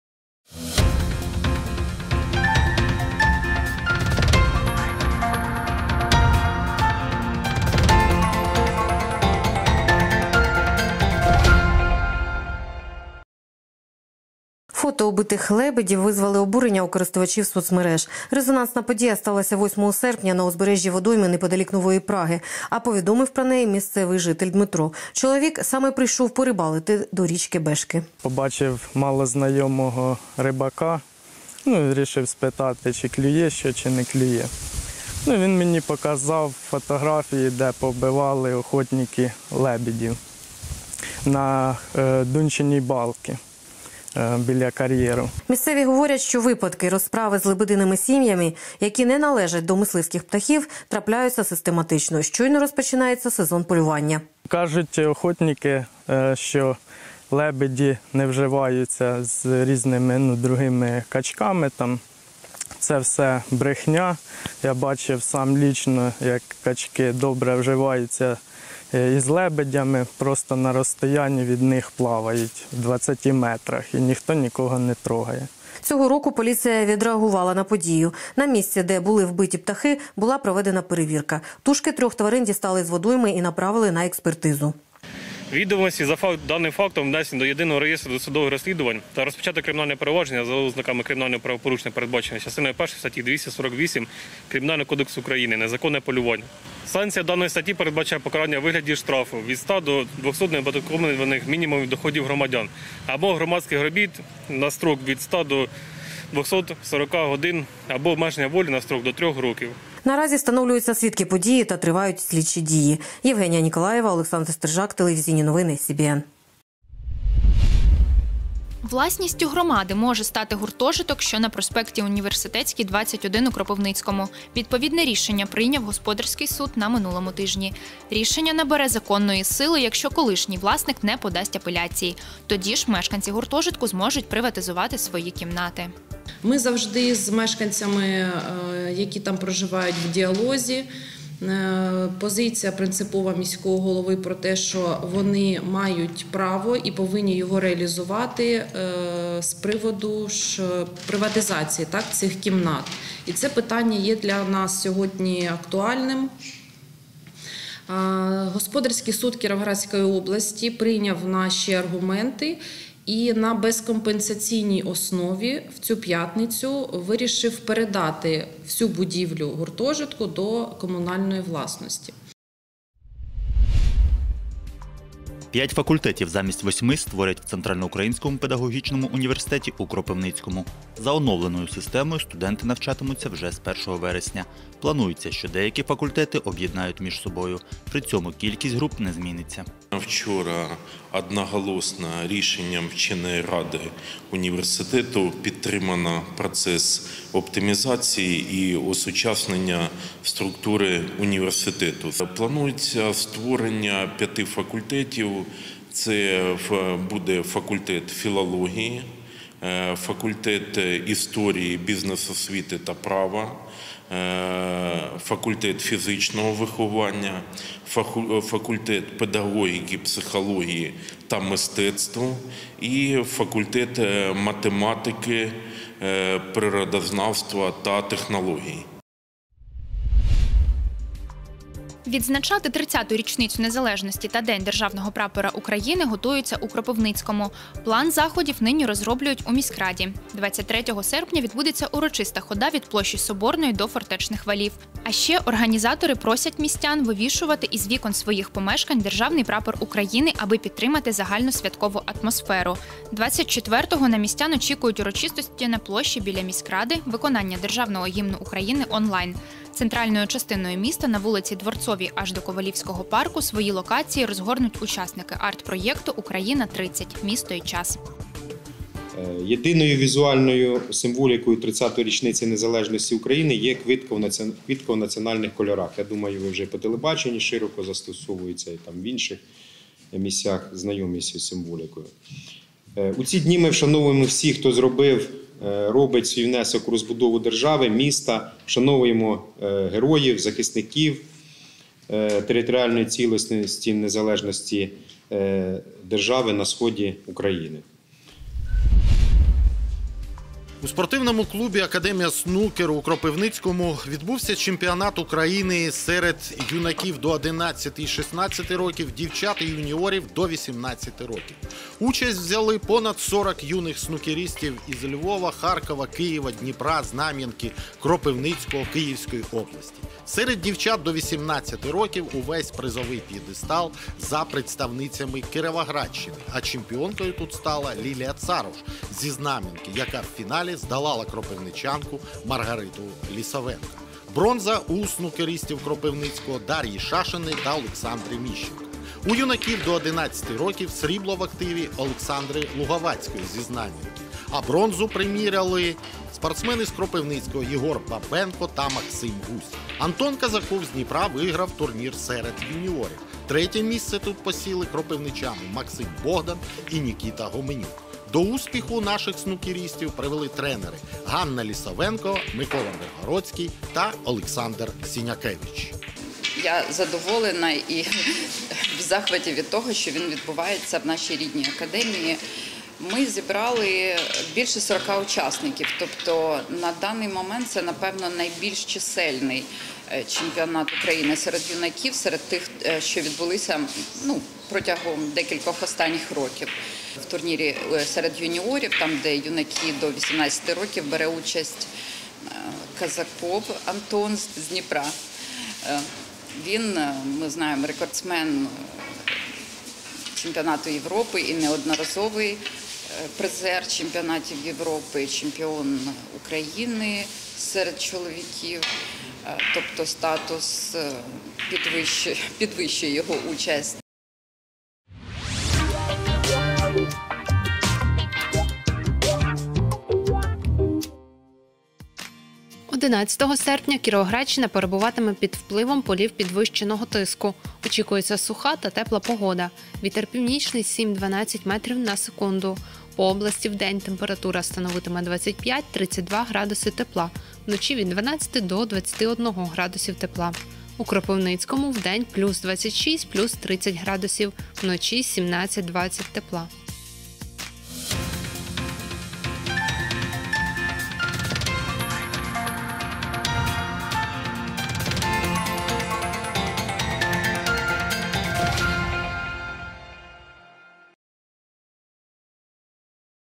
Головні новини Кіровоградщини | 10 серпня 2021 року | телеканал Вітер
- постріляли лебедів неподалік Нової Праги;
- власністю громади може стати гуртожиток, що на проспекті Університетський 21, у Кропивницькому;
- 5 факультетів замість 8 створять у Центральноукраїнському педагогічному університеті у Кропивницькому;
- відзначати 30-ту річницю незалежності та День Державного Прапора України готуються у Кропивницькому;
- Чемпіонат України зі снукеру серед юніорів відбувся у Кропивницькому;